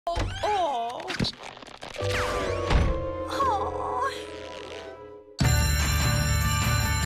Disney XD Latinoamerica • Sabatón se muda a Disney Channel (Marzo 2022)
Aqui el primer aviso sobre el cierre del canal...

Rec: 01/03/22